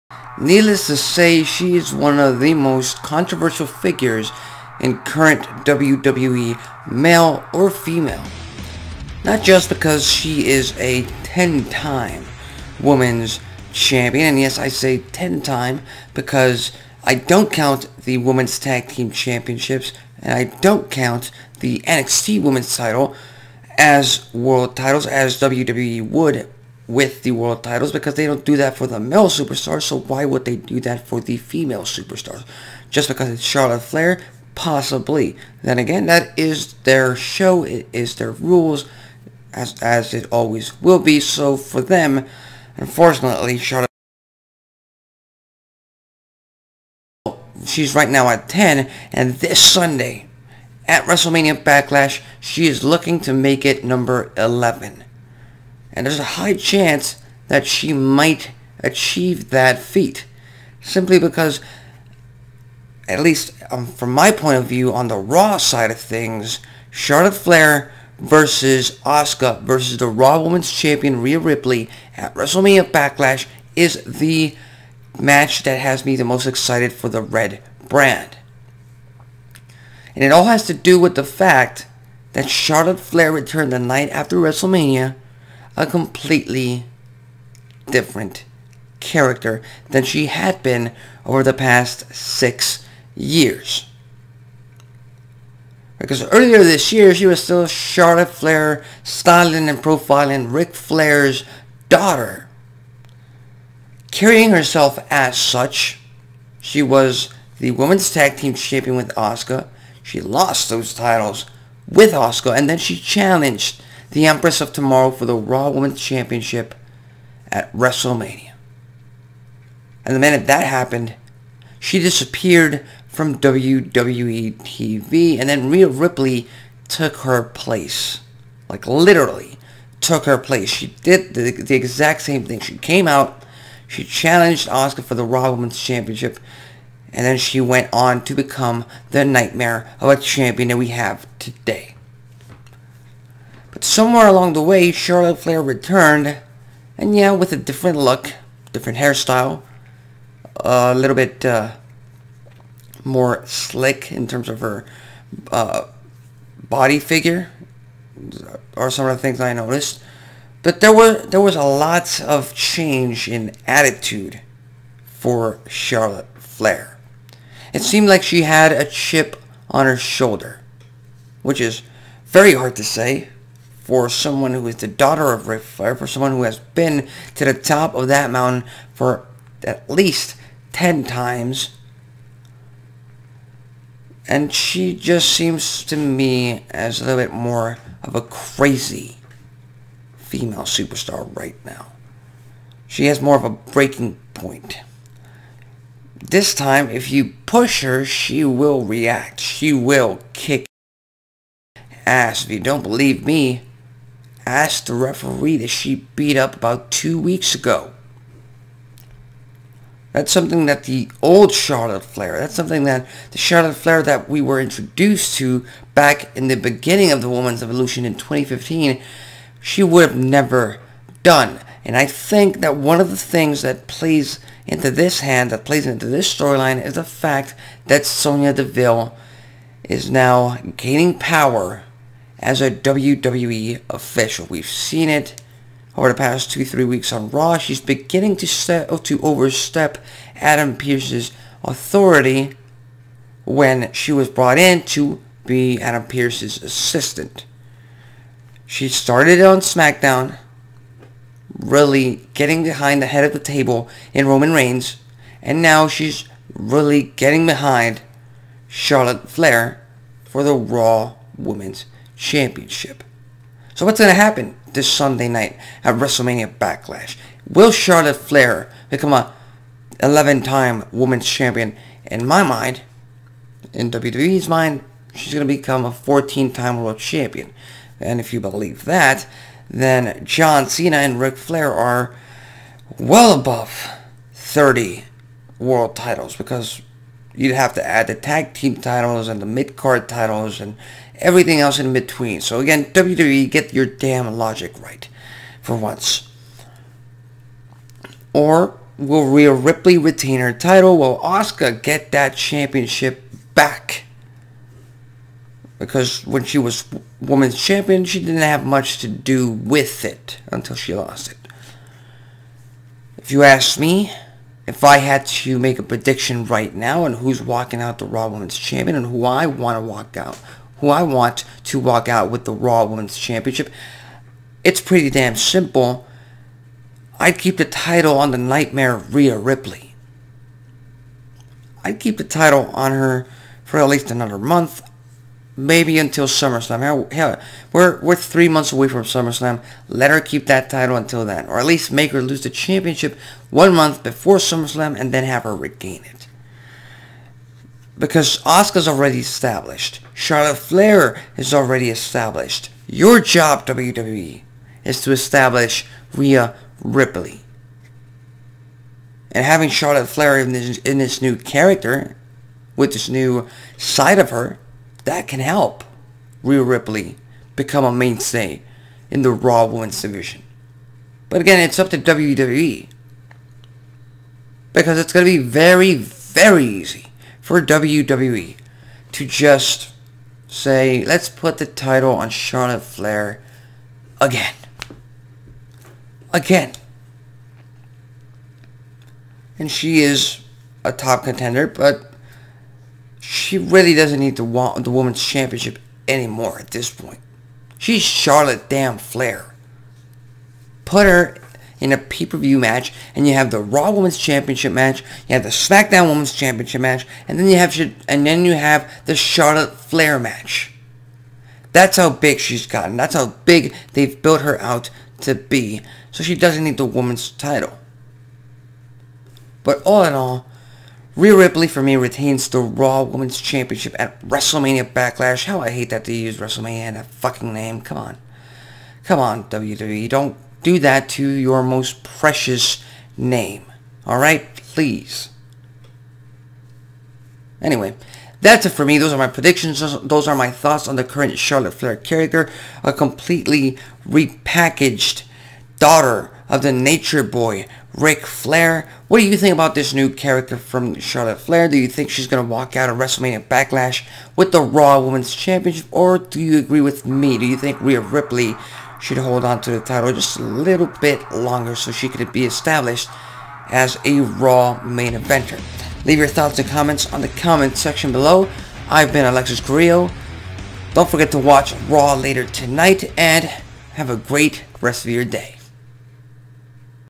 Let's Talk About Charlotte Flair - ACB Wrestling Talk
Alexis Carrillo gives his thoughts on the new presentation of the Charlotte Flair character on WWE Television. Her attack on a referee, her alliance with WWE Official Sonya Deville and the likelihood of her walking out of WrestleMania Backlash as the new RAW Women's Champion.

Leave your thoughts and predictions in the comment section below.

​Send your topic or question for ACB Wrestling Talk in email format to: